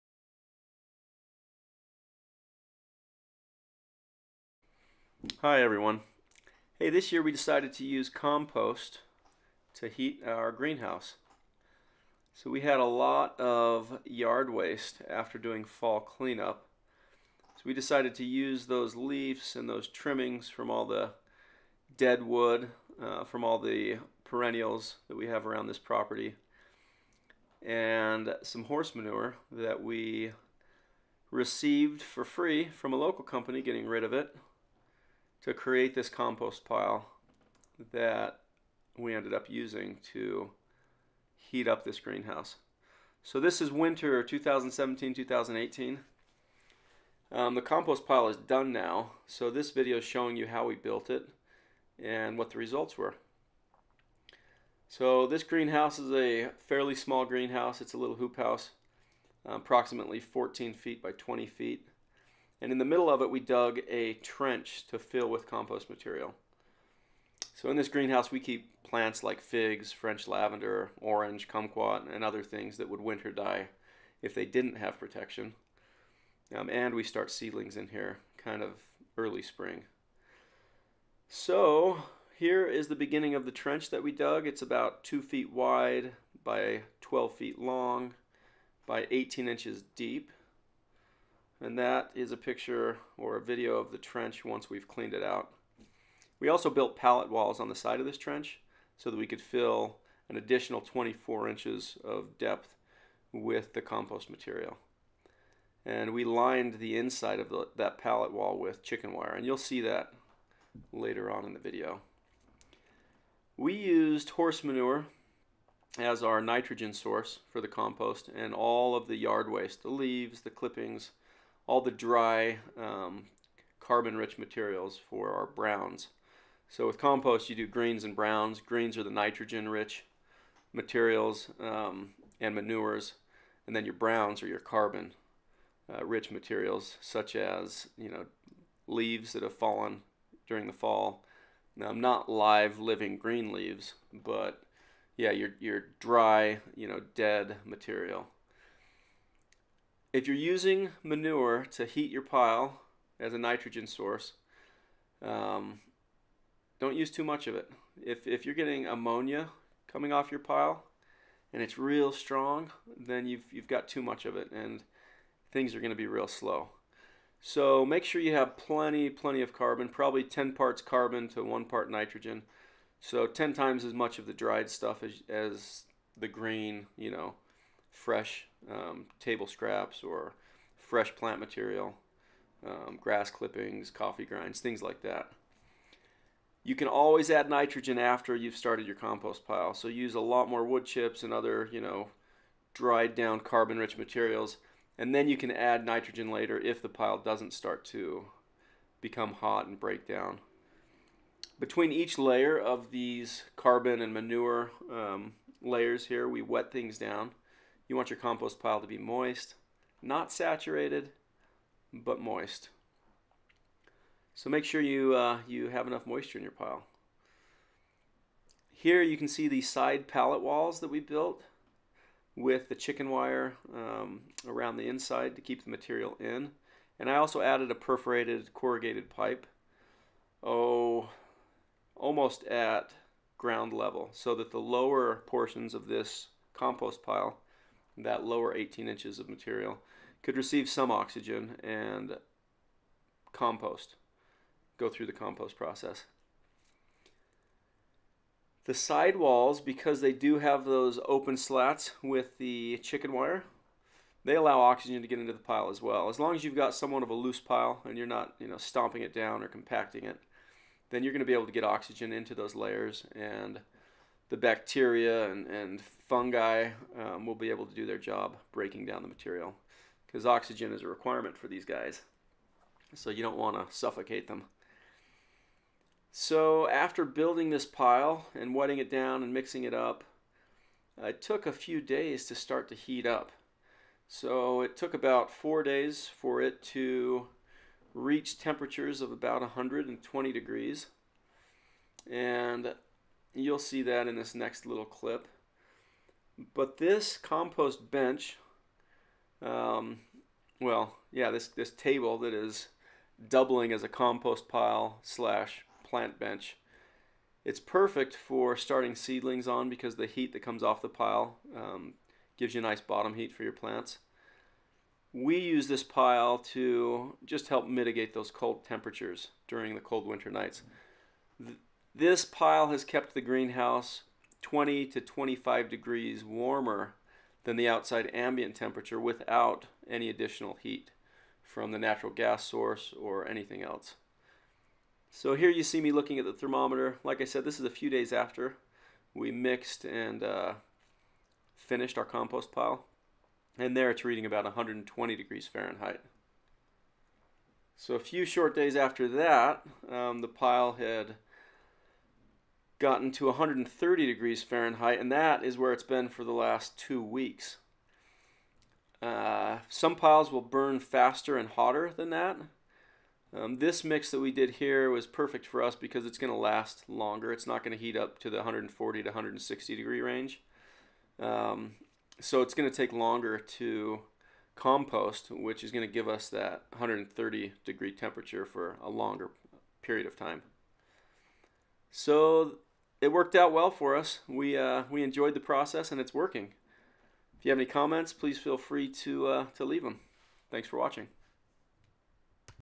Heating Your Greenhouse with Compost: How to
In this video I show you clips of me creating a compost pile in the middle of a cold frame (hoop house) greenhouse. Using compost to heat your greenhouse in the winter doesn't just give you a free alternative to heating your greenhouse, it creates CO2 in the greenhouse which helps your plants out.!